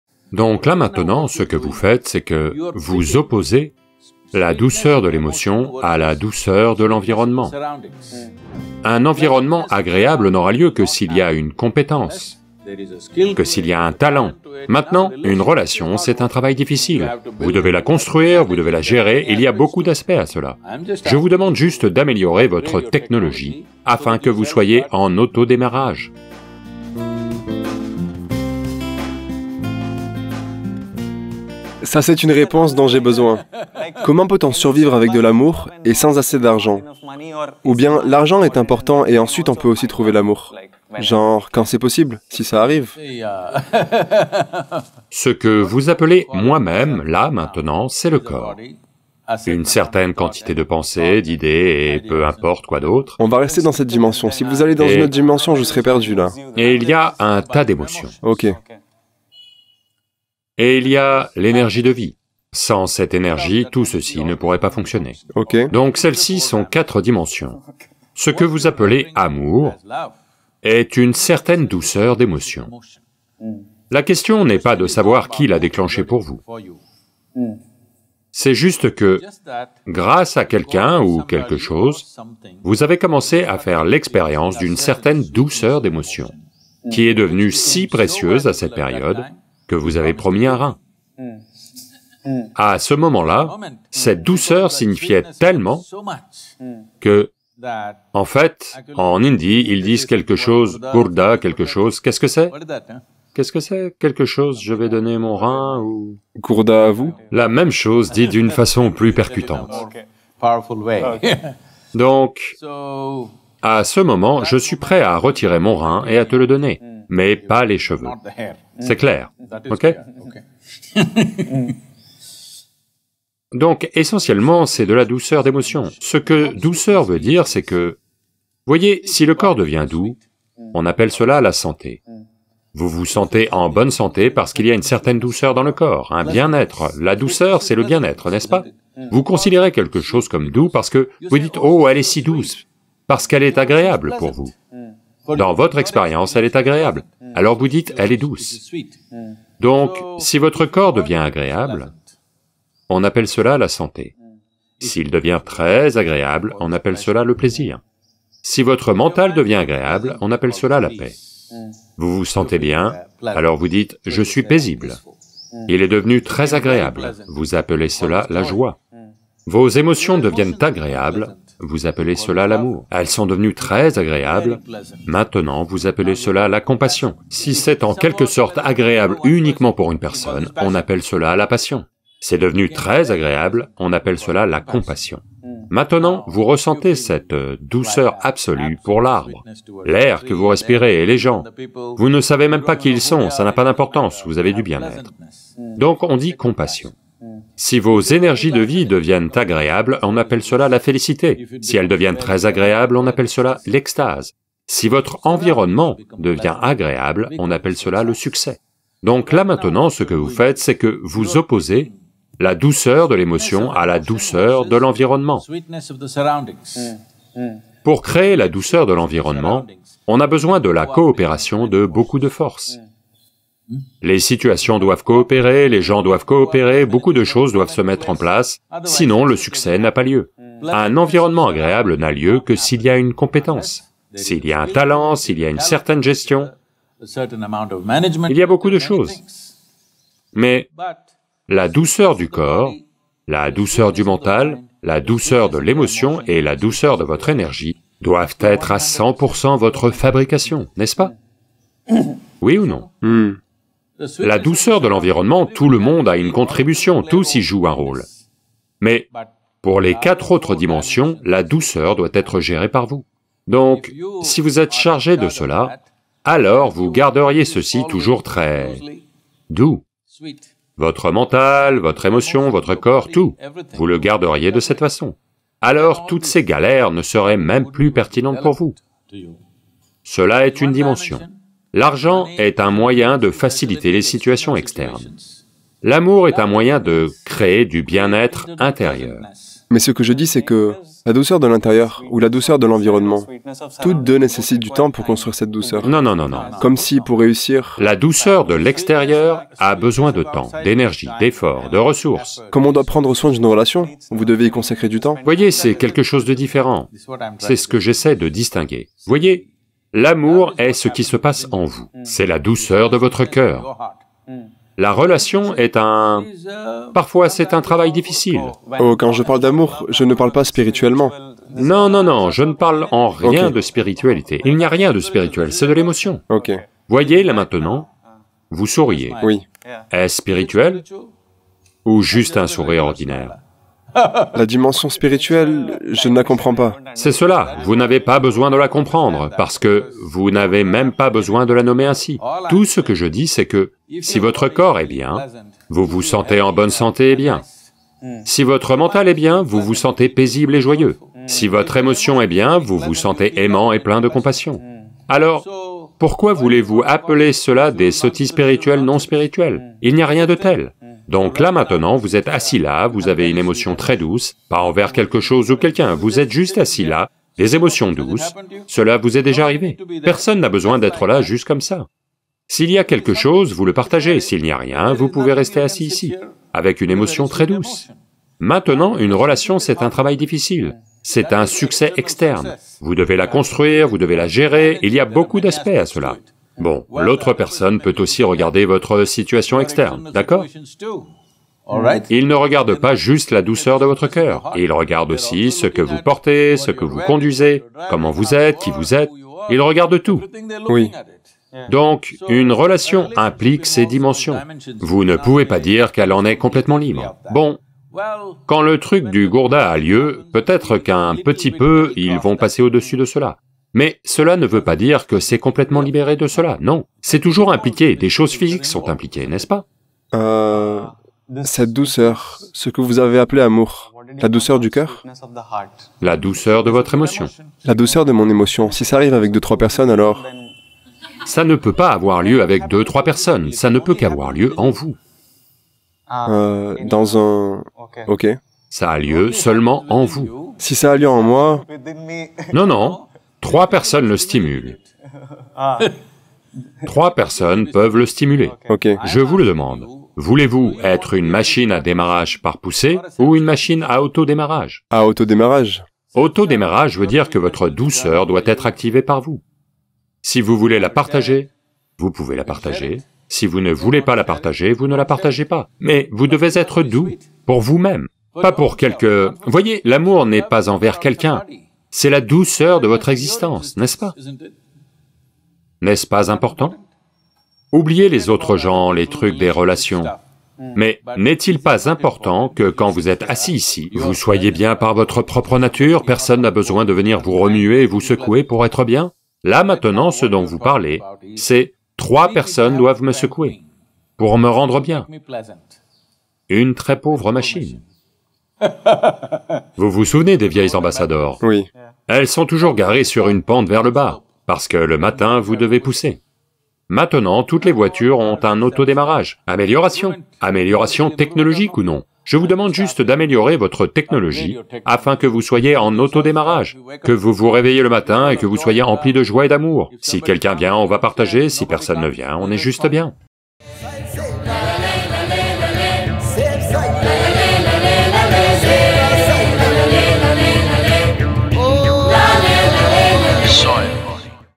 La vérité derrière l’amour et l’argent ?
Sadhguru répond à une question de l'acteur Vijay Deverakonda sur la gestion de l'amour et de l’argent dans la vie. Sadhguru fait la distinction entre l'amour et les relations, et explique la façon dont une relation réussie exige une gestion de nombreux ingrédients, alors qu'être aimant ne nécessite qu'un seul ingrédient.

🌸Inscription pour participer en tant que volontaire et inscription aux événements